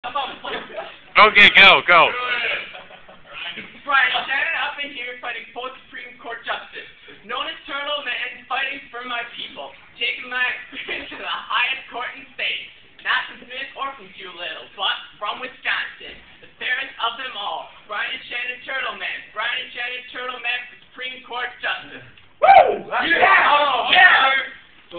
Turtleman Rap
Brian "Turtleman" Schanen laying down some sick lyrics at badger boys state